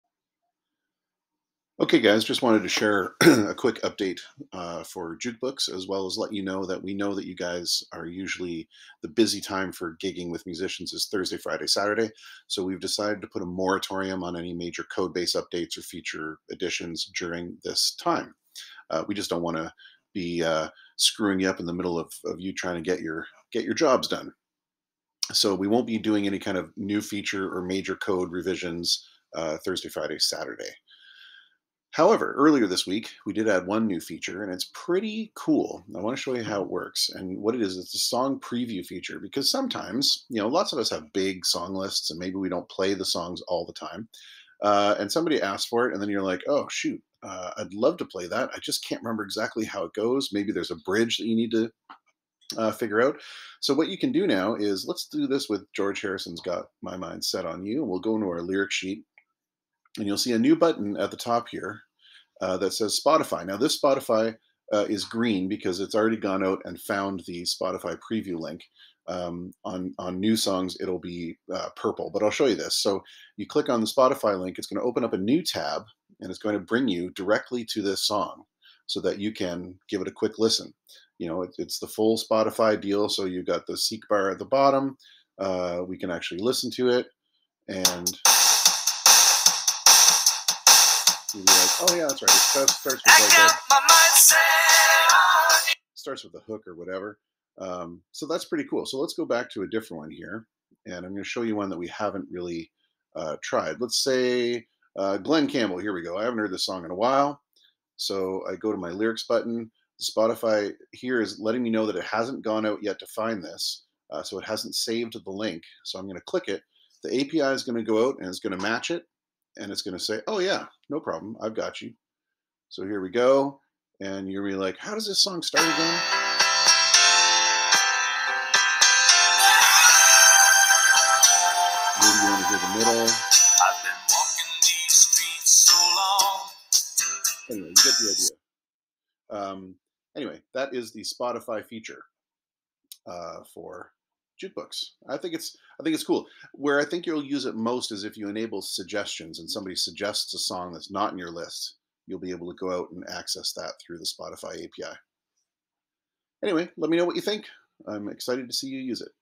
Introducing "SPOTBOT", song preview on Spotify.
New feature update - SPOTBOT, song preview on Spotify.
Also, we recognize that the busy gigging time for musicians is Thurs, Fri and Sat, so we're putting a freeze on major codebase changes or updates during these days ;)